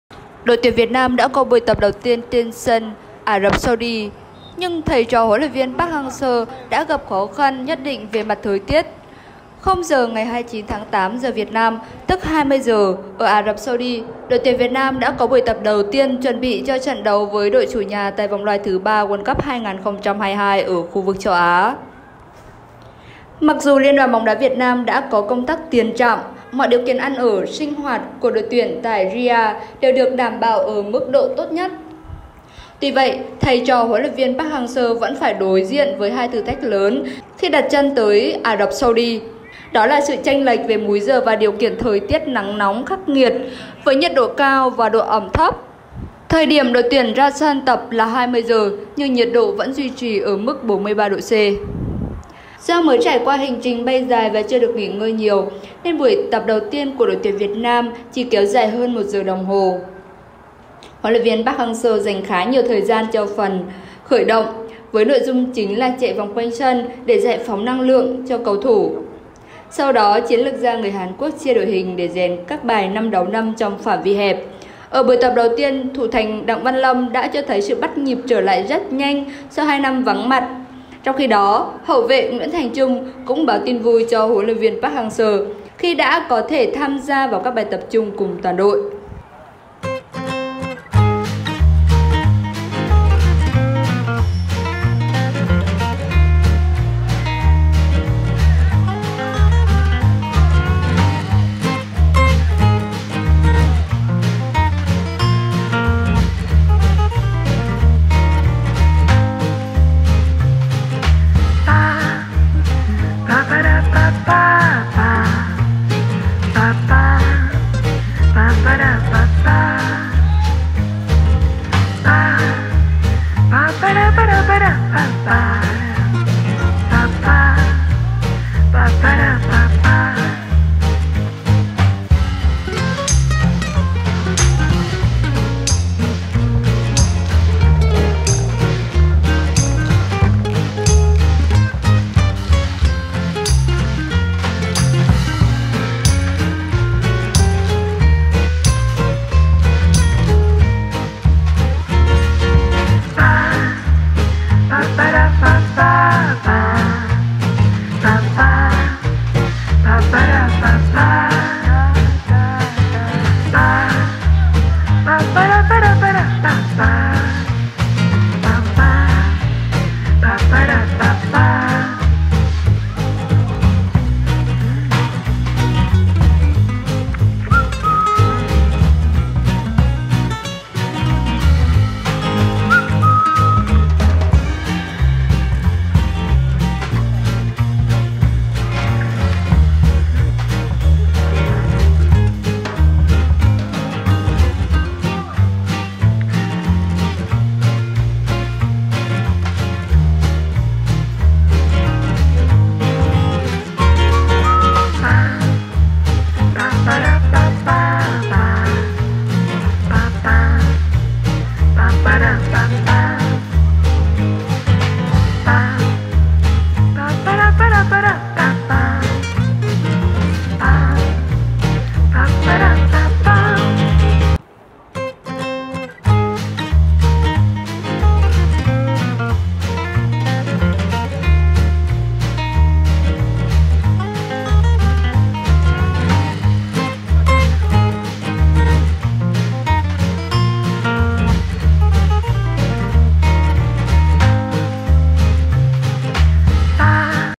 Đặng Văn hòa nhập với đội tuyển, các cầu thủ tập luyện dưới thời tiết nóng bức tại Ả Rập Xê Út
Tuyển Việt Nam có buổi tập đầu tiên tại Riyadh vào tối qua (28/8) để làm quen với điều kiện khí hậu và mặt sân tại đây.